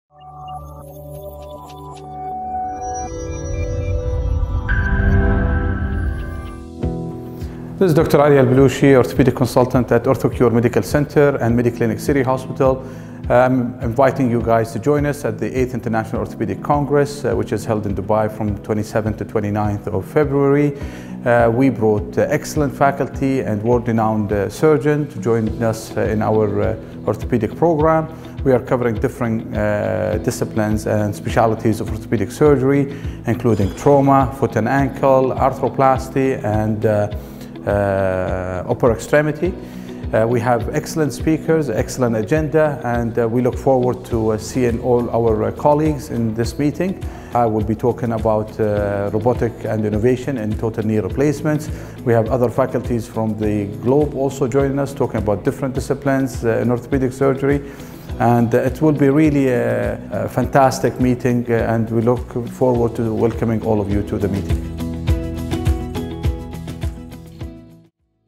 Emirates Orthopaedic Society VP, Asst. Prof. Ali Al Belooshi invites you to attend EIOC 2020!
Emirates Orthopaedic Society Vice President, Asst. Prof. Ali Al Belooshi cordially invites you to attend the EIOC 2020!

"We brought excellent faculty & world-renowned surgeons to join us on 27-29 February in Dubai during the EIOC 2020 covering the different discipline and speciality of orthopaedic surgery including trauma, foot and ankle, arthroplasty & upper extremities.”

Asst. Prof. Ali Al Belooshi will be lecturing on the 3rd day about

PFJ Ideal Candidate, tips, tricks and outcome - 08:30 - 08:40
UAE experience on Robotic assisted knee replacement - 09:10 - 09:20